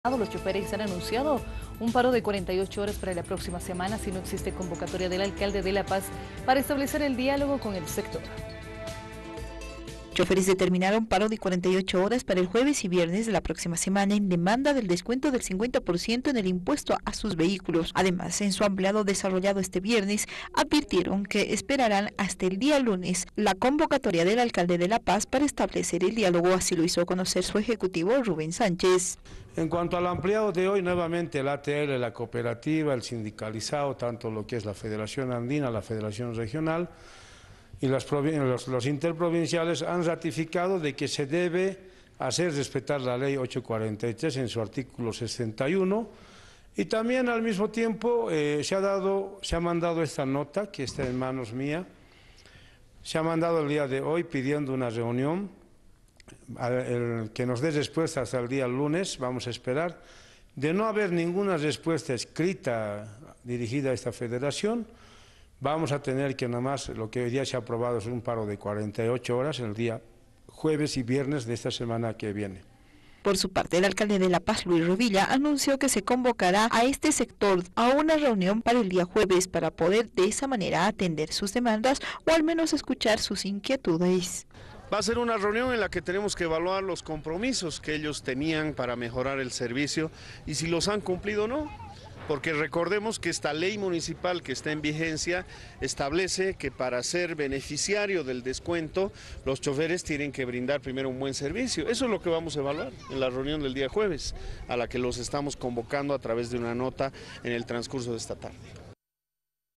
Transporte determina para de 48 horas para los días jueves y viernes- 21 oct 2016
(Abya Yala Digital).- Choferes anuncian paro de 48 horas para la próxima semana, si no  existe  convocatoria  del alcalde  de  La  Paz para establecer  el dialogo  con el sector.(video)